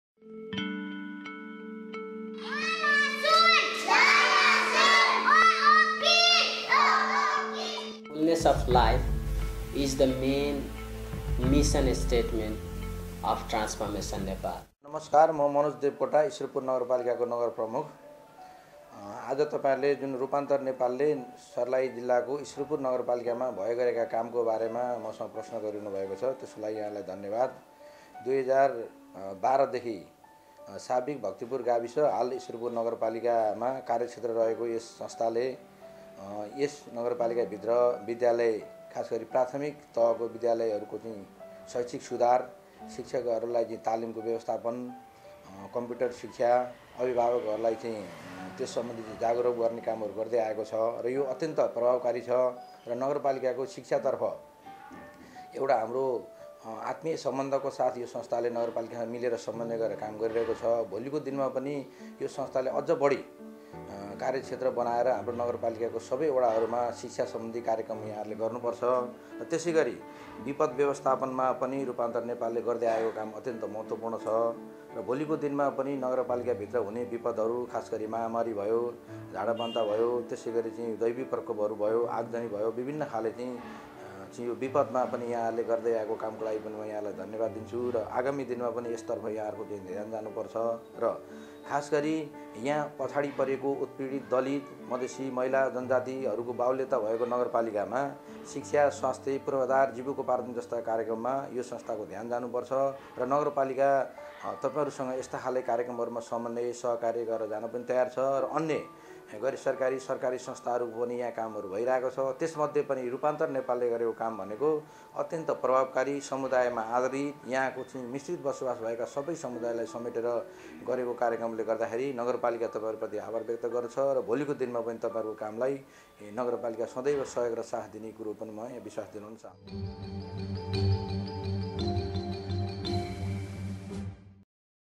Mr Manoj Kumar Devkota, Honorable Mayor, Ishworpur Municipality, Sarlahi
Feedback Message by Mr. Manoj Kumar Devkota, Honorable Mayor, Ishworpur Municipality, Sarlahi.